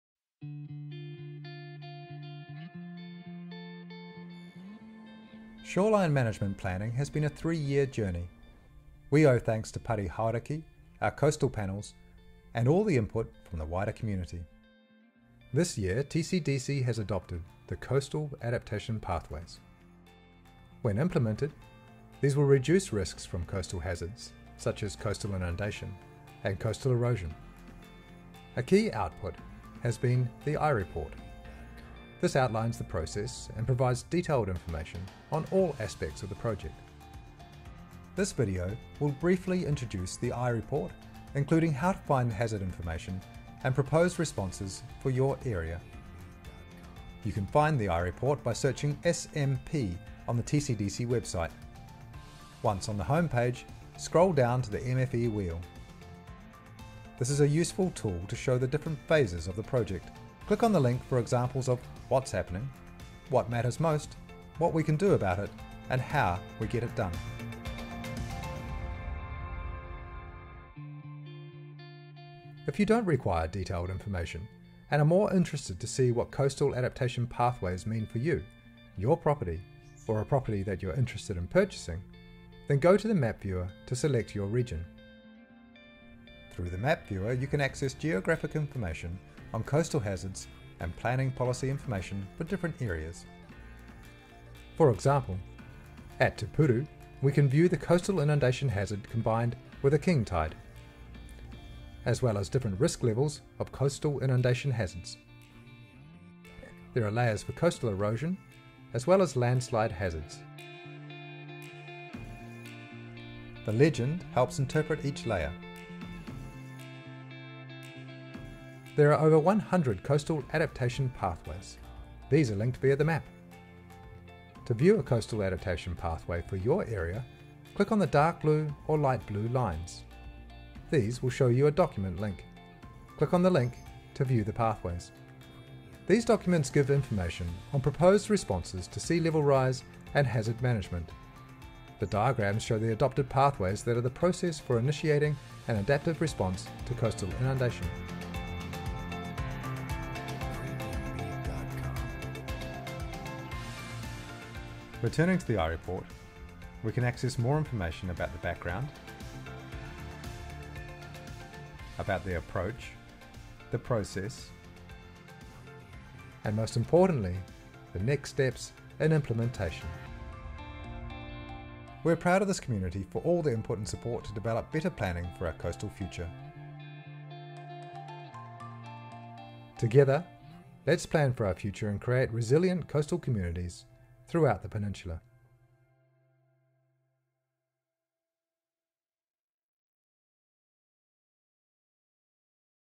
TCDC Shoreline Management Plan - how to navigate the ireport
Find the adaptation pathway for your area of the Coromandel's coast, as identified through our three-year shoreline management plan.